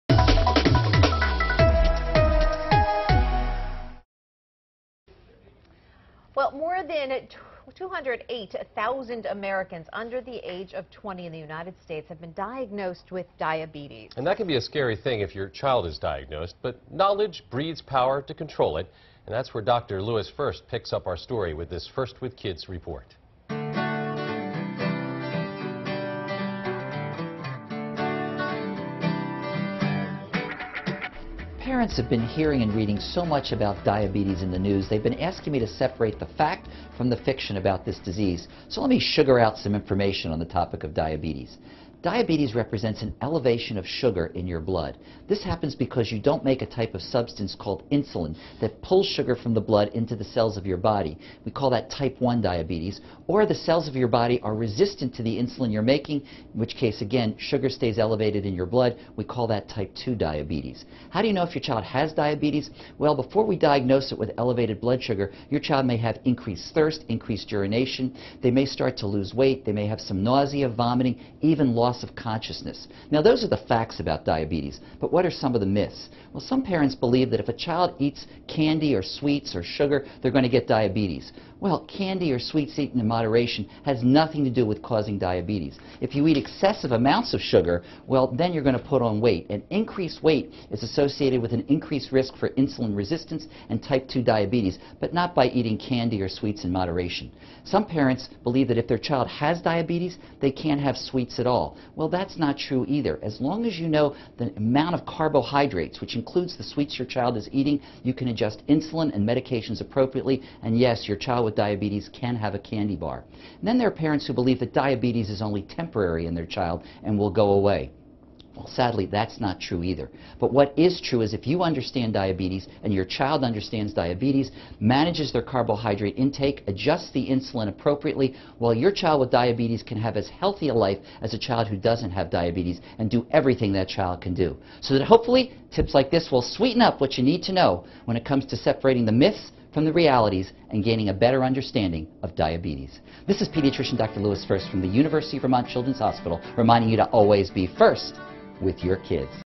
Separating diabetes facts from fiction
Dr. First separates some facts from fictions about diabetes.